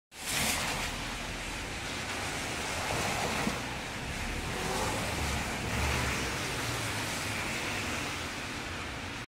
Mitsubishi EVO X Snow
On 3/10/17 snowfall starts up. Got a little bit of the Mitsubishi EVO X w a layer of snow on it. Share on Creative Commons, Twitter, Tumblr, Pinterest.